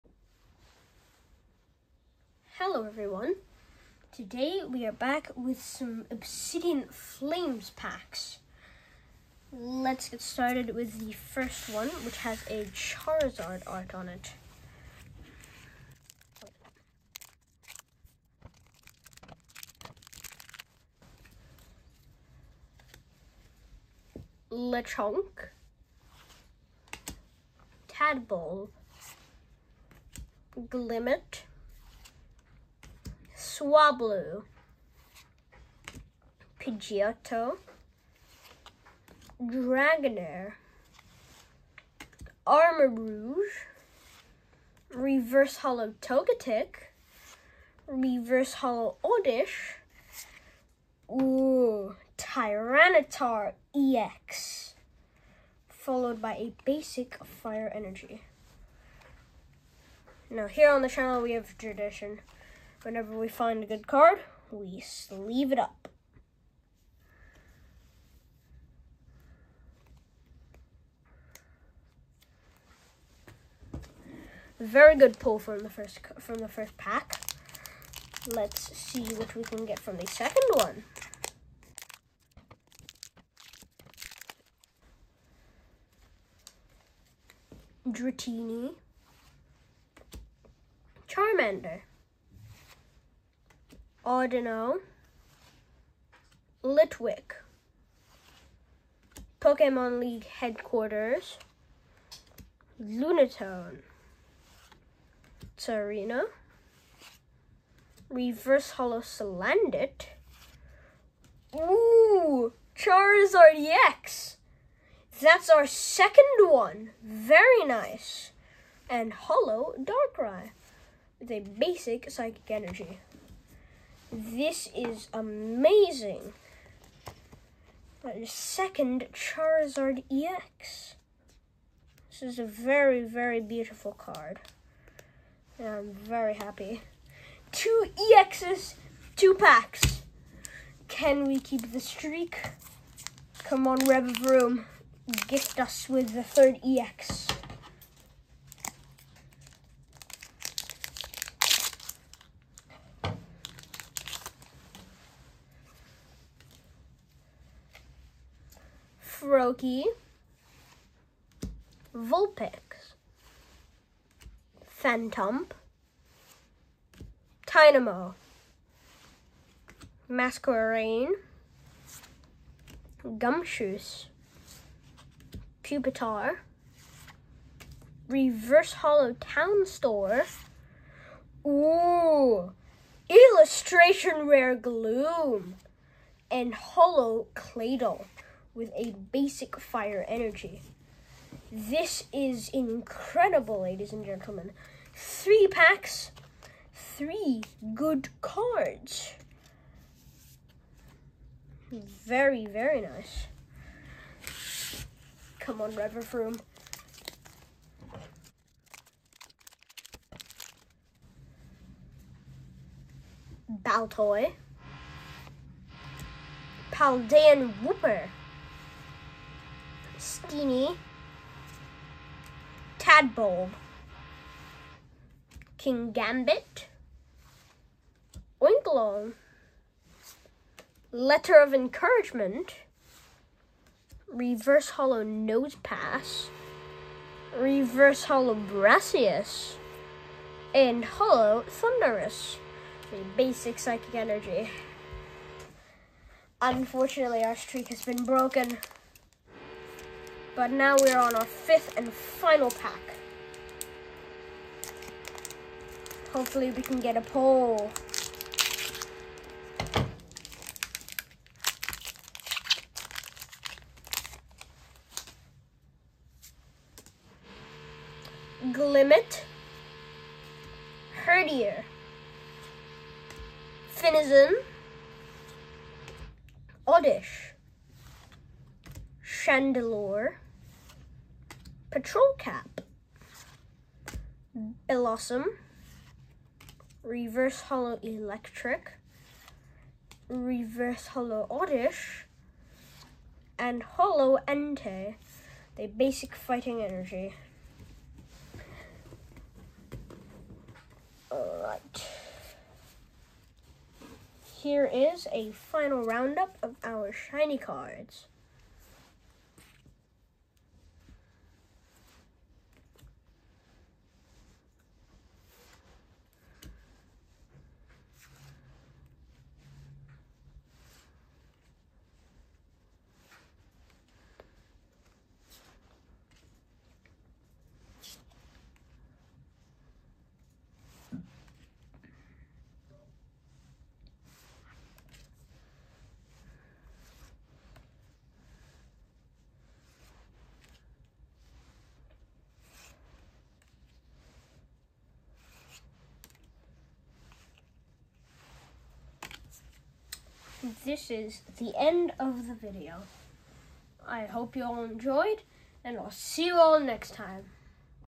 Very Nice Pulls From Obsidian Flames Boosters!
Pokemon TCG Scarlet & Violet: Obsidian Flames boosters opening.